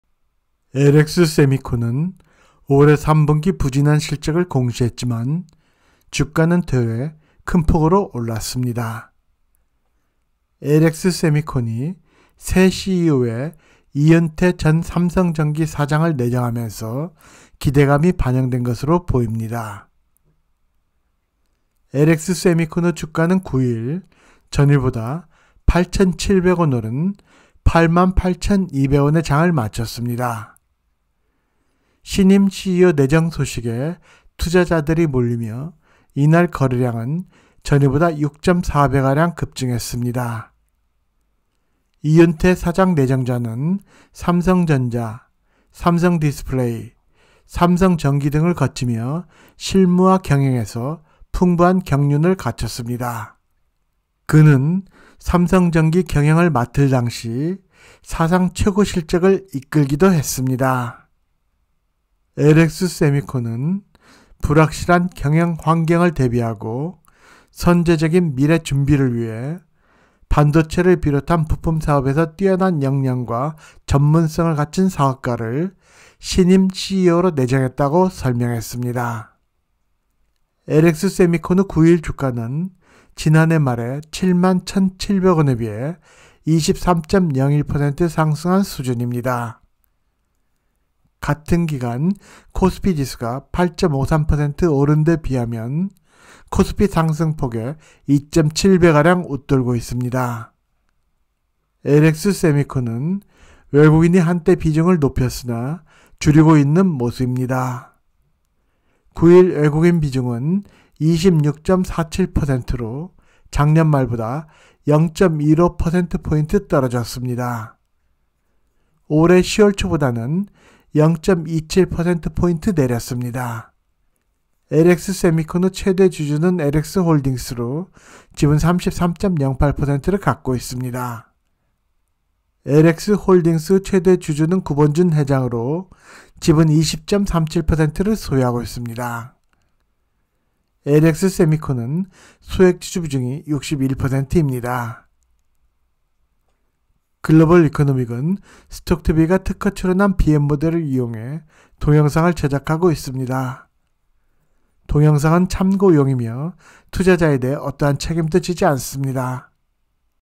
LX세미콘, 새 CEO에 주가 급등…이윤태 효과?
LX세미콘은 올해 3분기 부진한 실적을 공시했지만 주가는 되레 큰 폭으로 올랐습니다. LX세미콘이 새 CEO에 이윤태 전 삼성전기 사장을 내정하면서 기대감이 반영된 것으로 보입니다.
LX세미콘의 주가는 9일 전일보다 8700원(10.94%) 오른 8만8200원에 장을 마쳤습니다. 신임 CEO 내정 소식에 투자자들이 몰리며 이날 거래량은 전일보다 6.4배가량 급증했습니다.
이윤태 사장 내정자는 삼성전자, 삼성디스플레이, 삼성전기 등을 거치며 실무와 경영에서 풍부한 경륜을 갖췄습니다. 그는 삼성전기 경영을 맡을 당시 사상 최고 실적을 이끌기도 했습니다.
LX세미콘은 불확실한 경영환경을 대비하고 선제적인 미래 준비를 위해 반도체를 비롯한 부품사업에서 뛰어난 역량과 전문성을 갖춘 사업가를 신임 CEO로 내정했다고 설명했습니다.